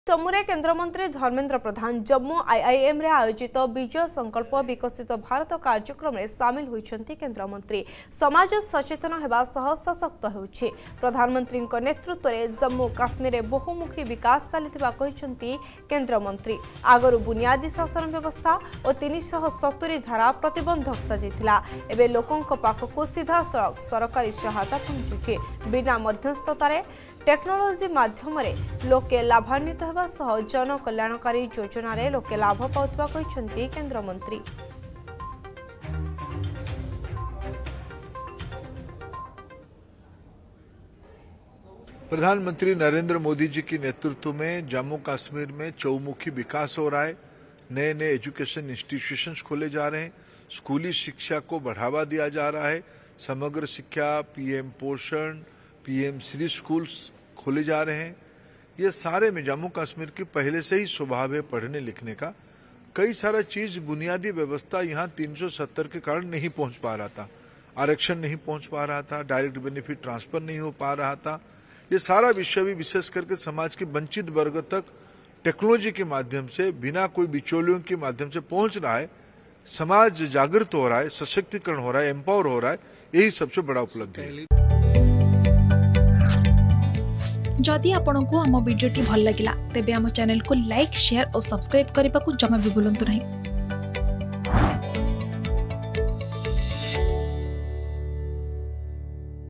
ଜମ୍ମୁରେ କେନ୍ଦ୍ର ଶିକ୍ଷାମନ୍ତ୍ରୀ ଧର୍ମେନ୍ଦ୍ର ପ୍ରଧାନ || Dharmendra Pradhan || Jammu
ଜମ୍ମୁରେ କେନ୍ଦ୍ର ଶିକ୍ଷାମନ୍ତ୍ରୀ ଧର୍ମେନ୍ଦ୍ର ପ୍ରଧାନ । ଜମ୍ମୁ ଆଇଆଇଏମରେ ଆୟୋଜିତ ବିଜୟ ସଙ୍କଳ୍ପ ବିକଶିତ ଭାରତ କାର୍ଯ୍ୟକ୍ରମରେ ସାମିଲ ହୋଇଛନ୍ତି କେନ୍ଦ୍ର ଶିକ୍ଷାମନ୍ତ୍ରୀ ଧର୍ମେନ୍ଦ୍ର ପ୍ରଧାନ ।